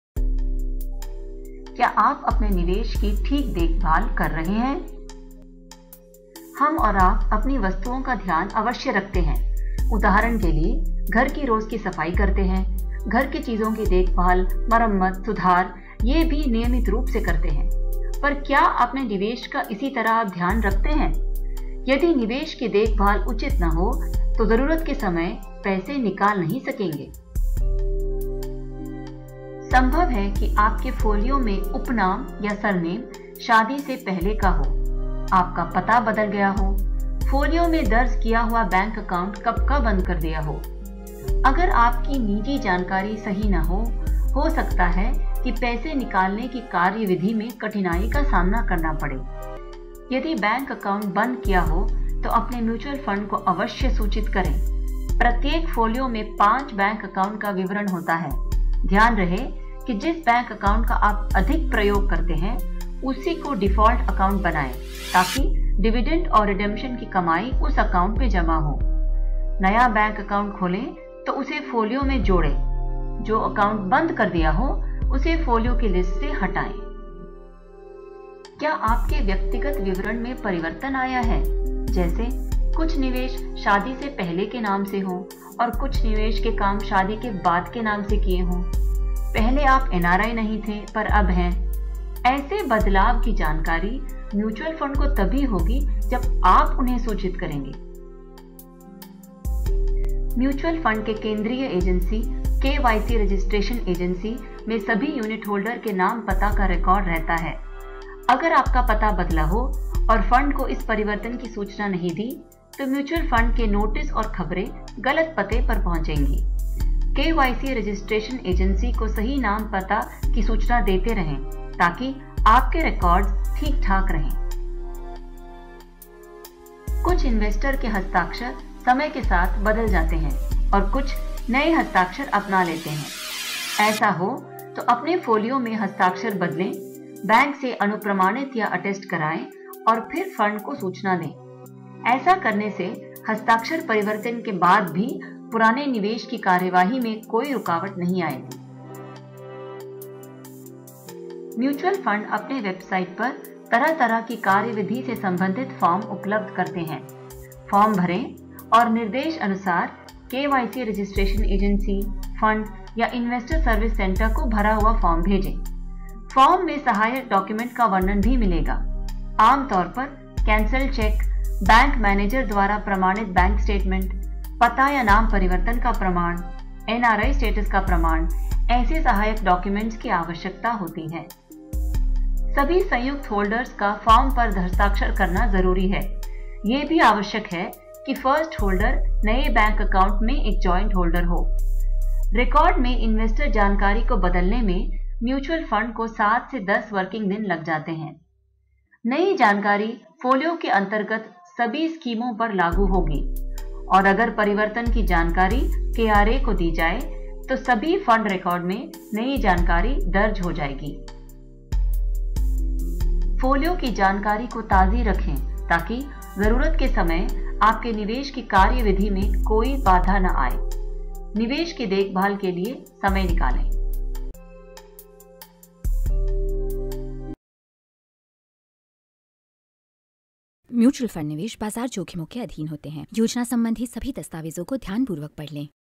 Maintaining your Investments (Hindi)
जिस प्रकार हम अपने घर का रख-रखाव करते हैं, उसी प्रकार हमारे निवेश को भी रख-रखाव की जरूरत होती है, अन्यथा आवश्यकता के समय हम उनका विवेकपूर्ण उपयोग नहीं कर सकते।

 आपके इंवेस्टमेंट्स की हाउसकीपिंग जैसे नाम, संपर्क, डाक पता, बैंक अकाउंट, आदि से संबंधित किसी भी जानकारी में अगर बदलाव आता है और वह आपके फोलियो में अपडेटेड नहीं है, तो वह आपके लेनदेन में रूकावट बन सकता है।

 जब भी आपके व्यक्तिगत विवरण में परिवर्तन होता है, तो म्यूचुअल फंड हाउस को सूचित करें ताकि वे इसे फोलियो में अपडेट कर दें।

 इसे और विस्तार में समझने के लिए यह वीडियो देखें।